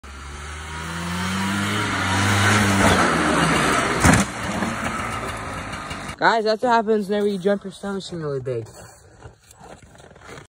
Jumping snowmobile with no suspension (bad idea?)
Jumping my old broken snowmobile with no suspension. Bad idea?

⚠️WARNING⚠️
Do not try any of this at home. The following stunts are performed by professional stunt dummies.￼